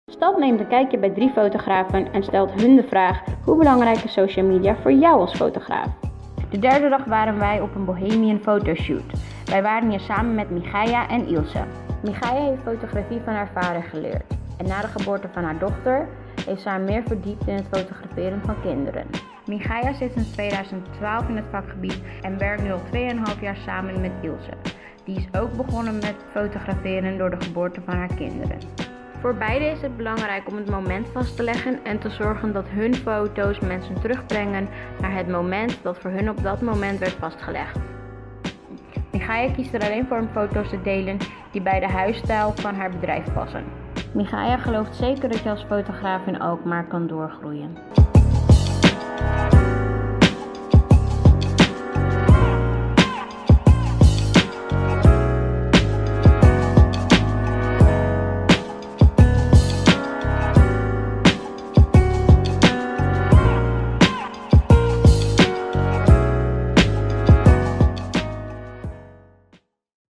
Behind The Photo: Michaja Ezri Wilken
Wij zijn deze maand een nieuwe thema ingegaan. Tijdens dit thema zijn wij een dag wezen meekijken met drie fotografen uit Alkmaar. Elke fotograaf kreeg dezelfde vraag gesteld: Hoe belangrijk is social media voor jou als fotograaf?